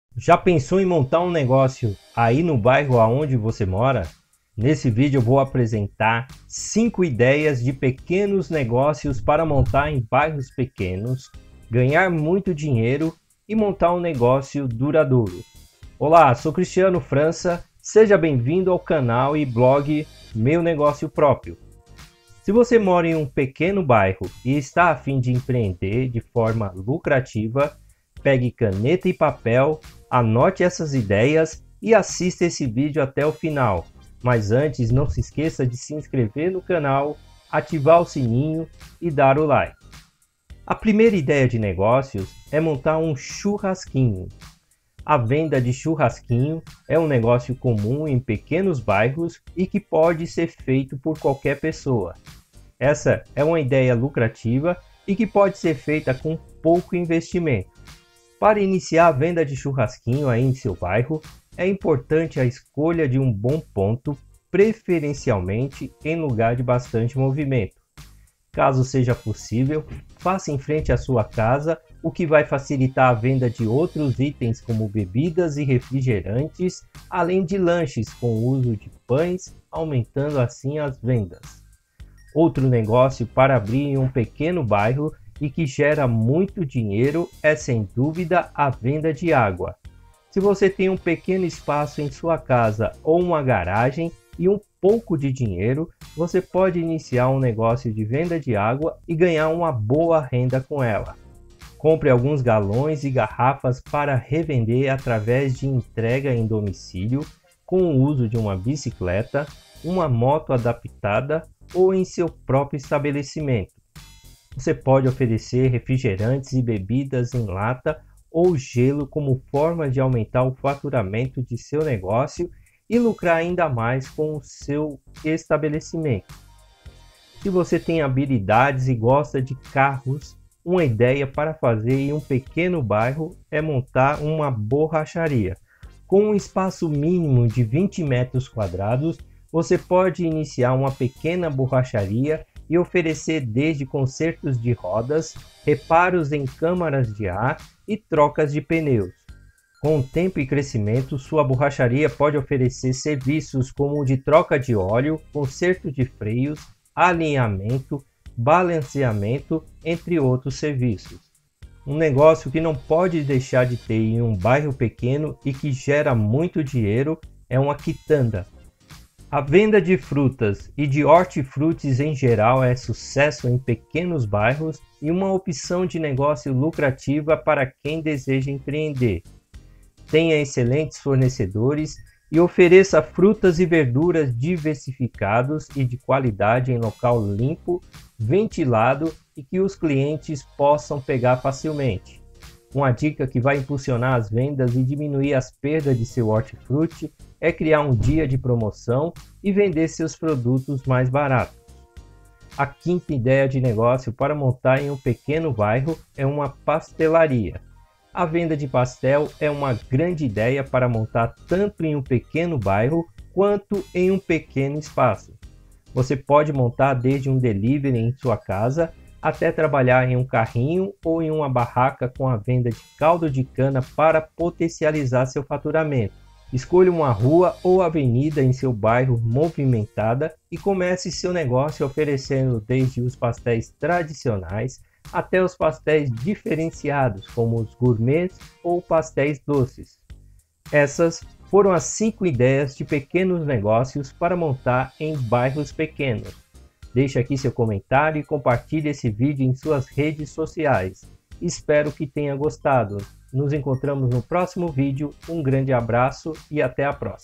5 Ideias de PEQUENOS NEGÓCIOS Para MONTAR Em BAIRROS PEQUENOS
5 Ideias de PEQUENOS NEGÓCIOS Para MONTAR Em BAIRROS PEQUENOS
👉Link CURSO MEGA PASTEL DELIVERY🔽🔽

5 Ideias de PEQUENOS NEGÓCIOS Para MONTAR Em BAIRROS PEQUENOS

Para quem está atrás de Ideias de negócios, nesse vídeo apresentamos 5 Ideias de Negócios Para Montar em Bairros Pequenos que podem ser feitos com pouco investimento.

A ideia de abrir um negócio em um bairro pequeno sempre foi algo que intrigou as pessoas, pois a maioria acredita que é difícil fazer sucesso e ganhar dinheiro com qualquer que seja o empreendimento.

Porém, é bom que se dica que nos bairros pequenos há muitas oportunidades, já que em muitos casos os moradores preferem comprar suas coisas ali mesmo, o que estimula a economia e demonstra o potencial que pode ser aproveitado.

Mas os candidatos a empreendedores ficam em dúvida do que montar e como abrir um negócio realmente duradouro nessas regiões. O primeiro passo para abrir um negócio em um bairro pequeno é se planejar e analisar o que está faltando para, assim, começar a amadurecer a ideia.

Veja a concorrência, o dinheiro que você tem disponível e o que realmente se enquadra com você e assim inicie essa ideia, mas assista a esse vídeo já que ele tem ideias que podem ser feitas em pequenos bairros e que você pode aproveitar.